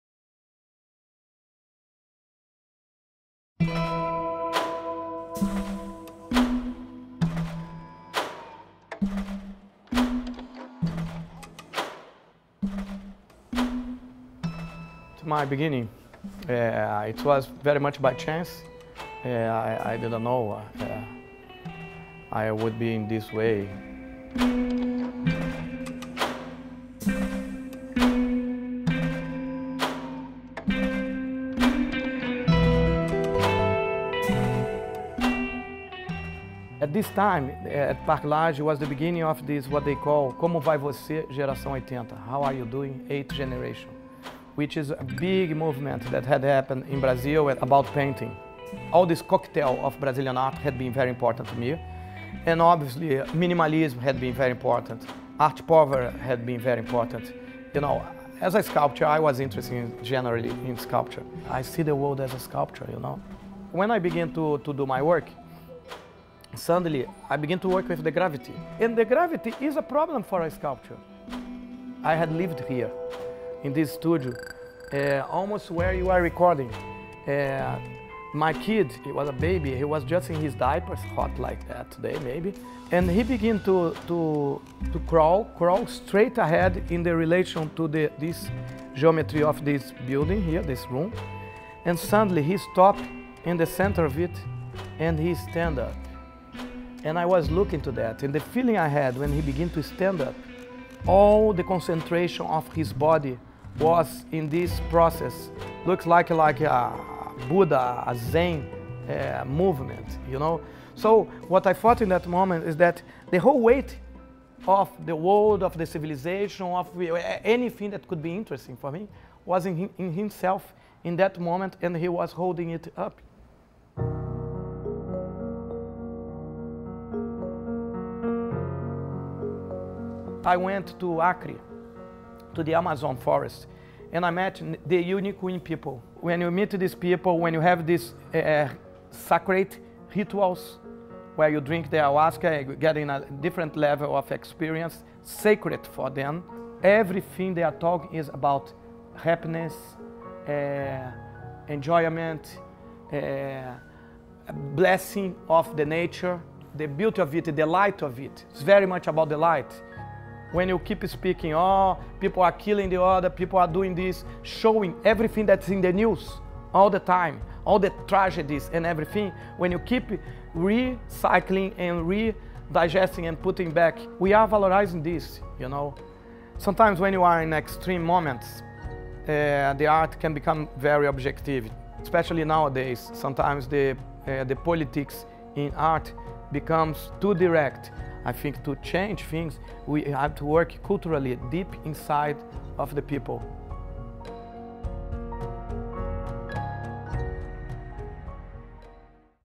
Ernesto Neto
Swing from politics to ayahuasca with one of the leaders of Brazil's contemporary art scene.